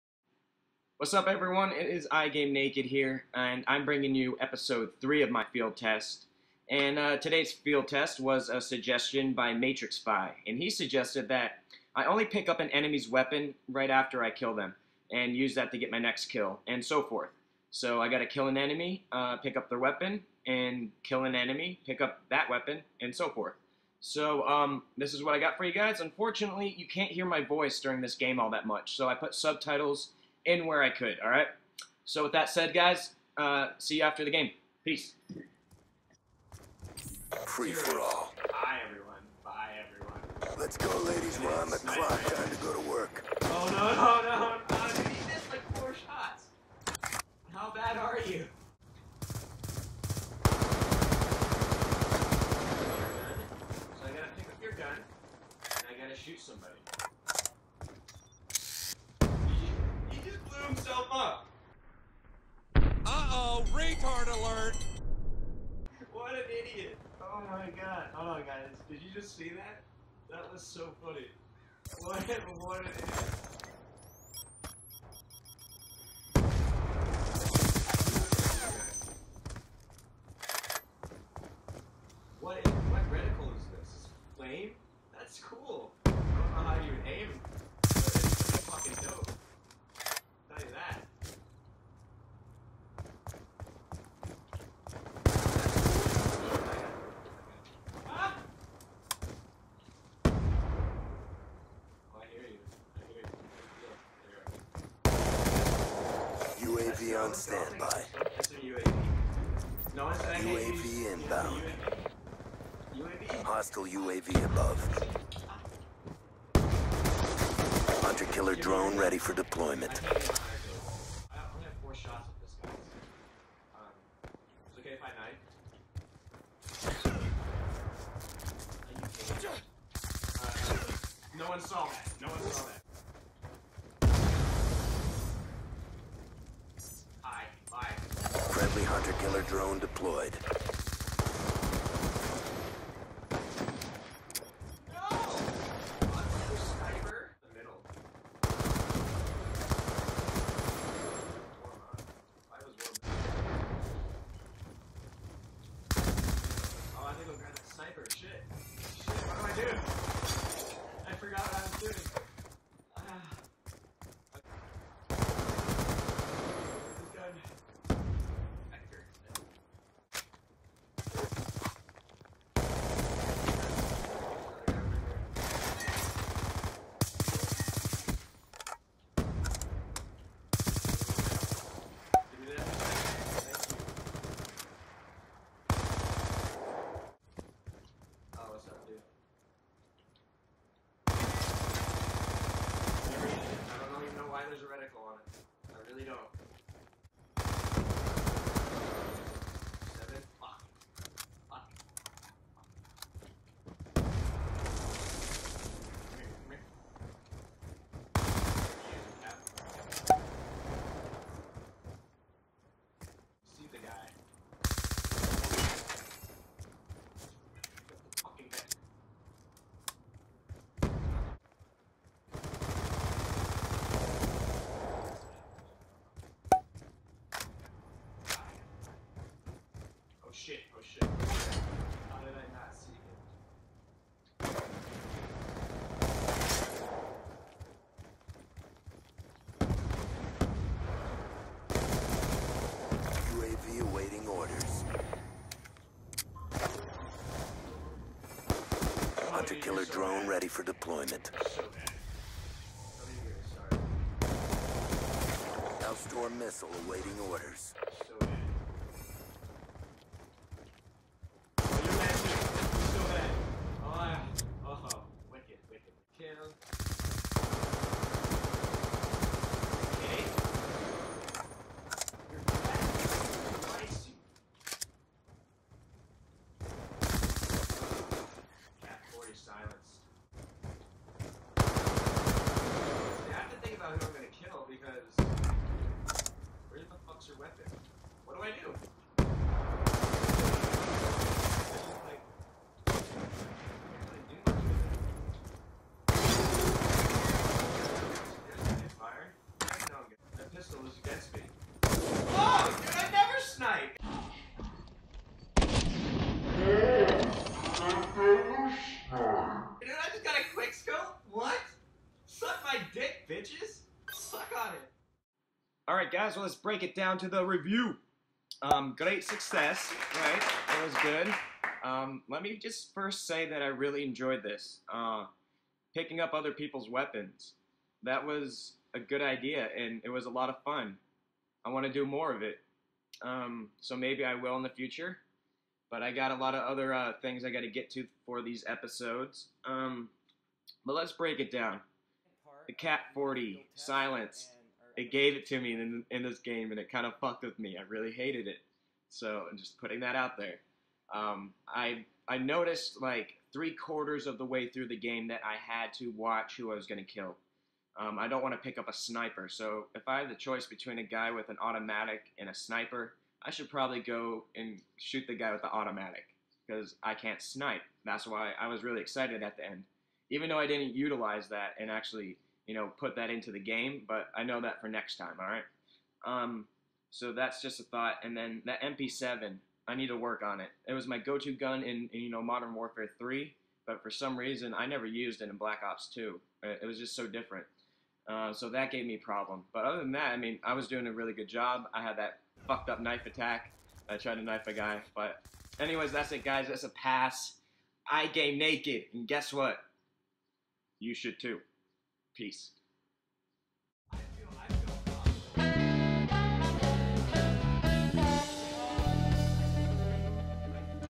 Field Test - Episode 3 - Picking Up Enemy's Weapons
Episode 3 - A comment suggestion by Matrixfy. Pick Up An Enemy's Weapon After Killing Them, And Use It To Kill Your Next Opponent. You Can't Hear My Voice All That Well. I'm So Sry About That!